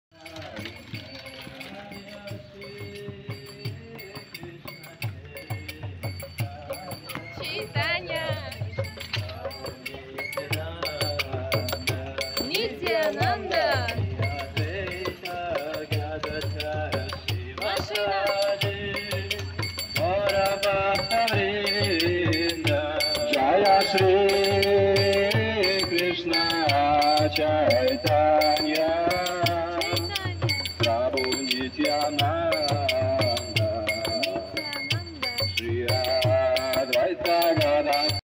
Харинама на Камчатке 16.07.2023 / Harinam in Kamchatka 07/16/2023
Место - Петропавловск-Камчатский
Лидер - Е.М. Киртида дд

🎥VIDEO ABOUT SANKIRTAN !!!

Location - Petropavlovsk-Kamchatsky
Leader - H.G. Kirtida dd

🙌🏻PARAM VIJAYATE SRI KRISHNA SANKIRTANAM.